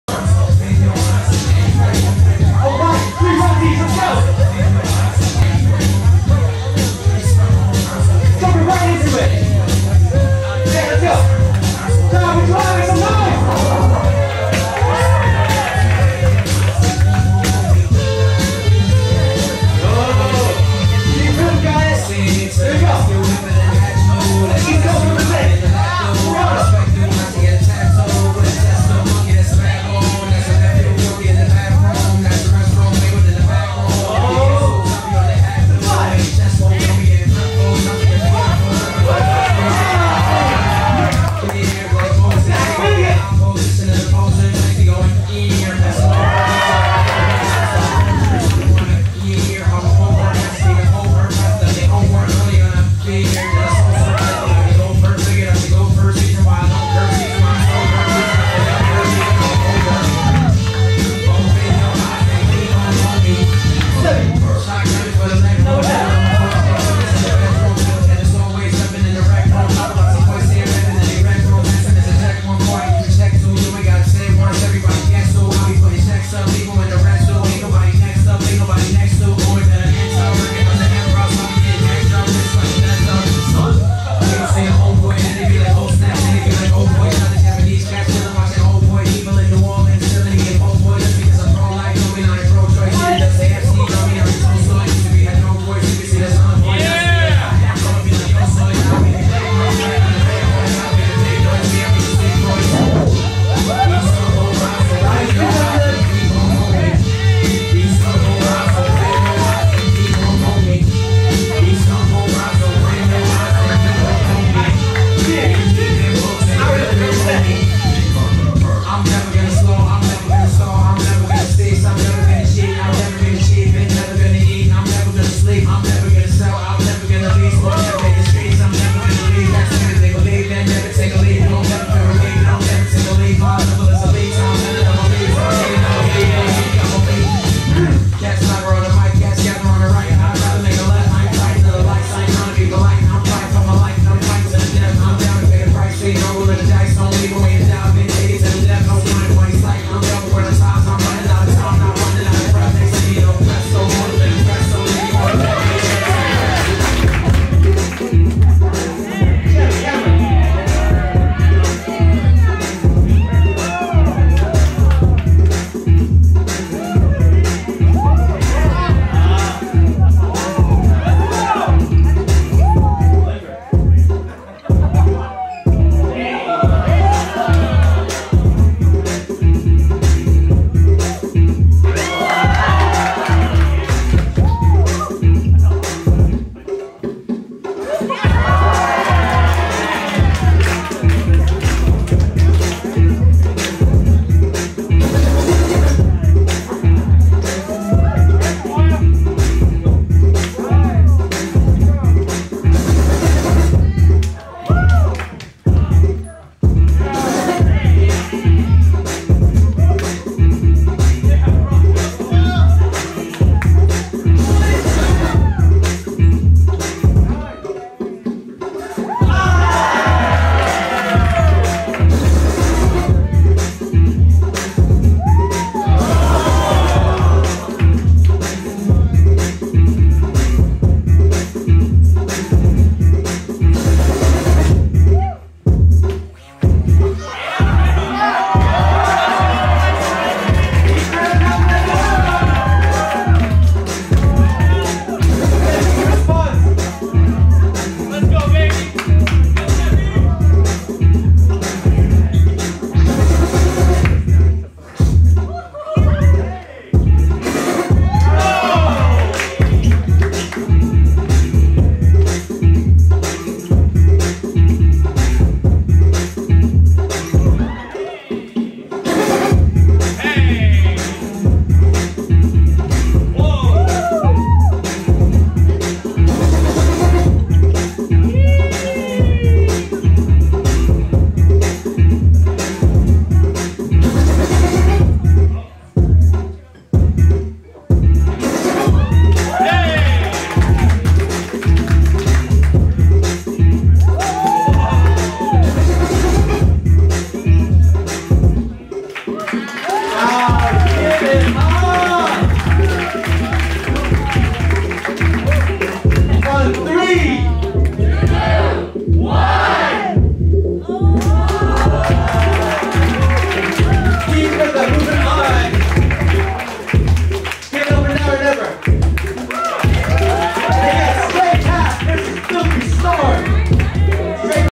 Deep Rhythm vs Now or Never | Top8 | UDC 2v2 Break the Halls Vol.2
UBC Unlimited Dance Club Presents:

Break the Halls Vol.2, 2v2 Breakin' Battle
December 10, 2021 @ UBC AMS Nest

DJ Jaylo & Kai

Judges:
Complex [Hoodz, Dance Broomz]
Straycurt [Sray Path]
Artattacks [Floor Storm]